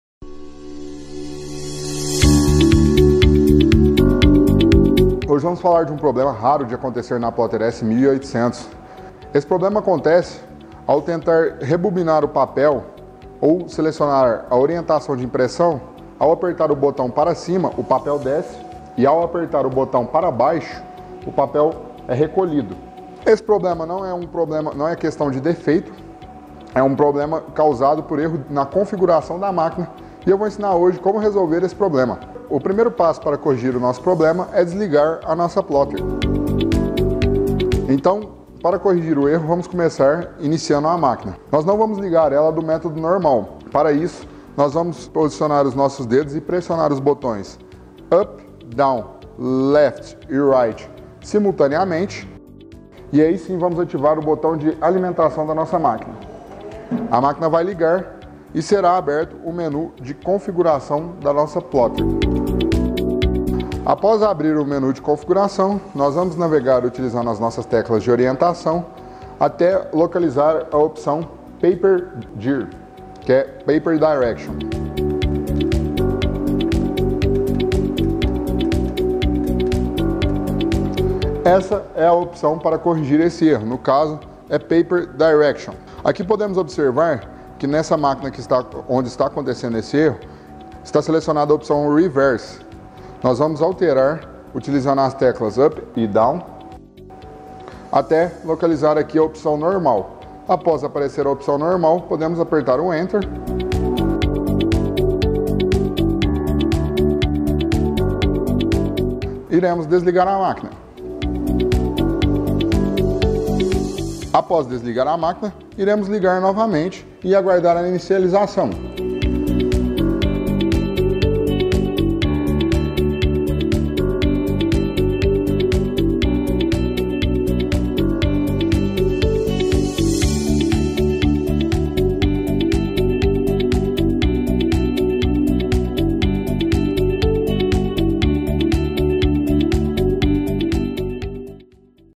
Multivisi | Visutec | Configurando a direção do papel - Plotter S1800
Veja no vídeo acima, como corrigir um erro de configuração da  Plotter S1800 da Visutec.
Este erro consiste em ao apertar o botão para baixo, a plotter recolhe o papel para dentro e ao apertar o botão para cima o papel é liberado da máquina.
Após realizar este passo a passo, sua máquina estará pronta para o uso.